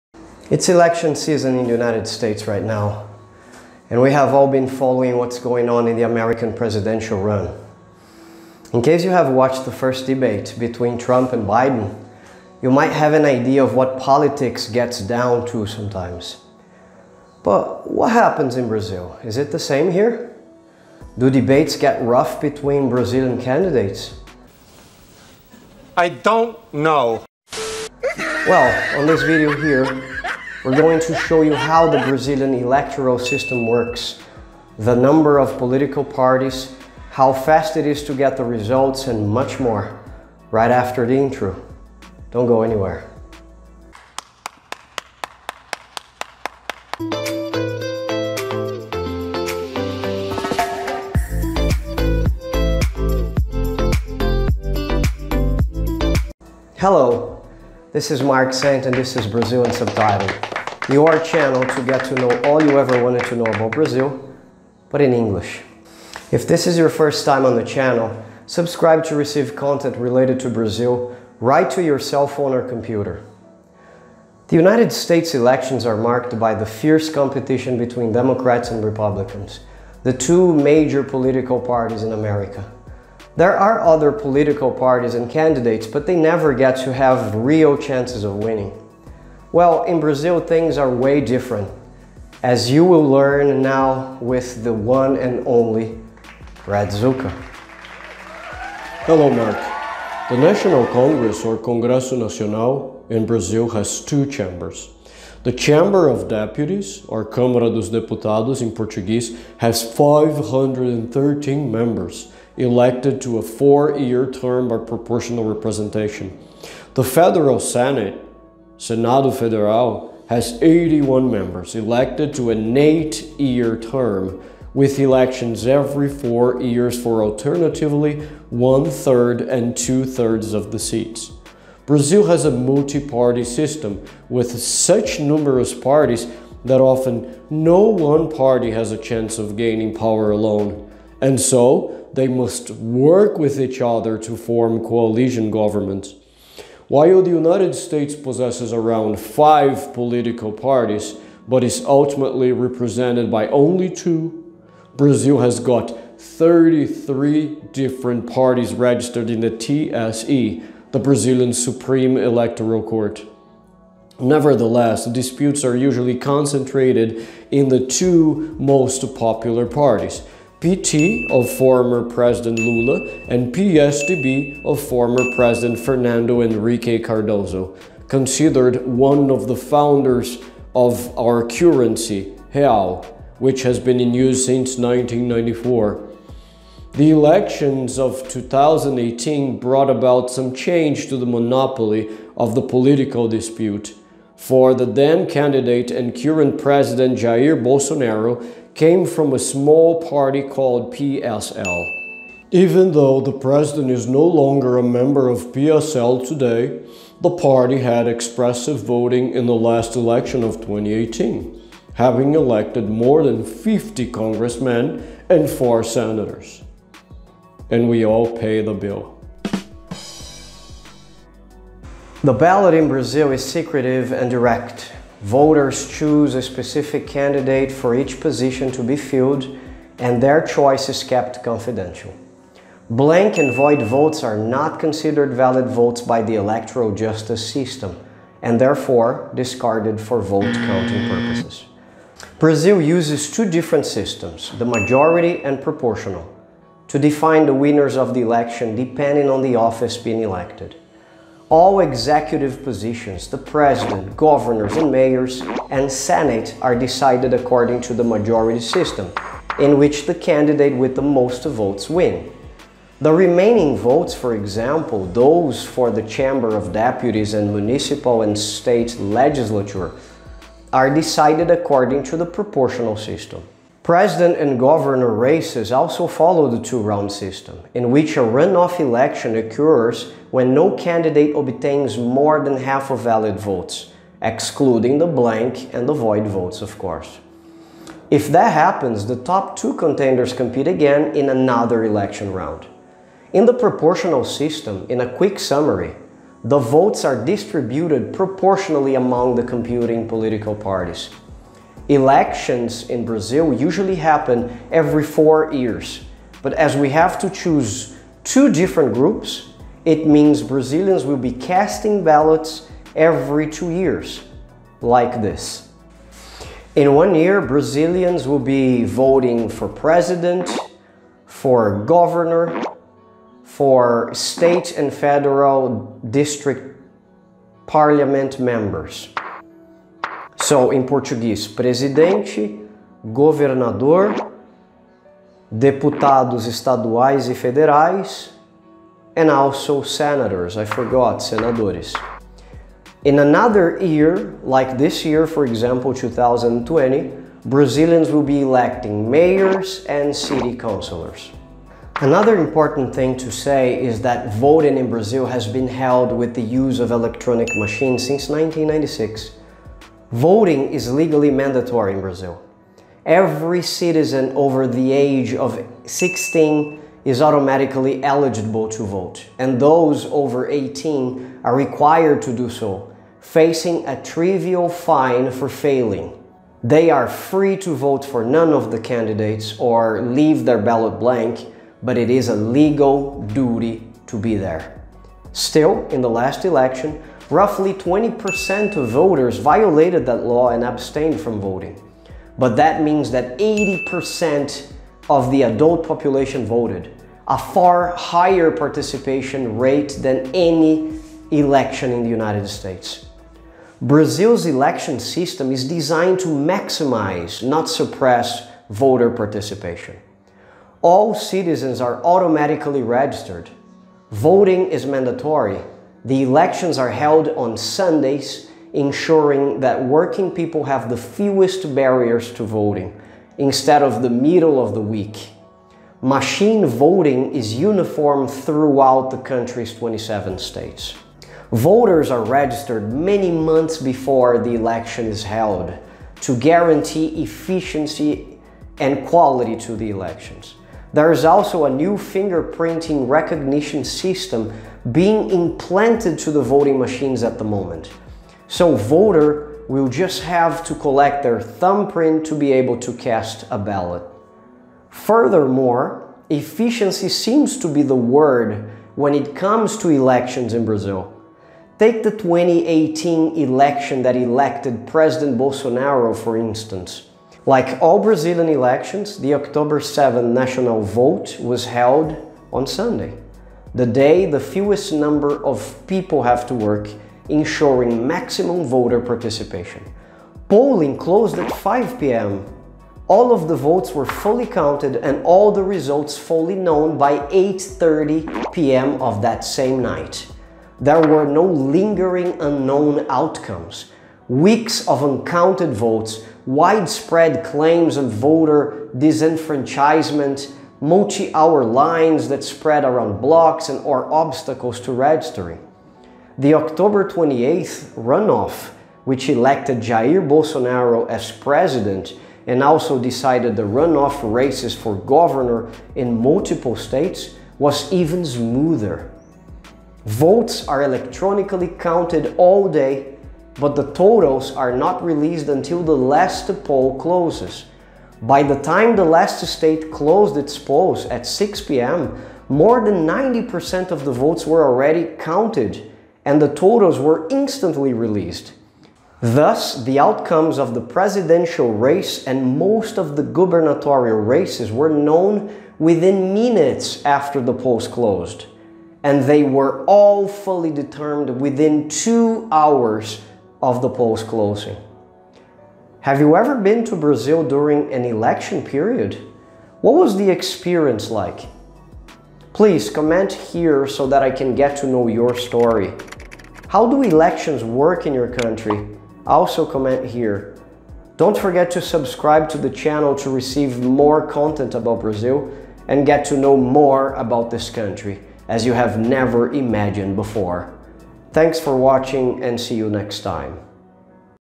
BRAZIL'S HIGH-TECH ELECTORAL SYSTEM - One of the best in the world!
Elections in Brazil are probably one of the most efficient in the whole world. Not only it is meant to maximize electors' participation, but also has a bullet-speed vote-counting system. In this video, you will learn everything about how our electoral system works and be prepared to understand and follow this year's elections in Brazil. Enjoy!

TELEGRAM: Brazil Unsubtitled – Tips from Brazil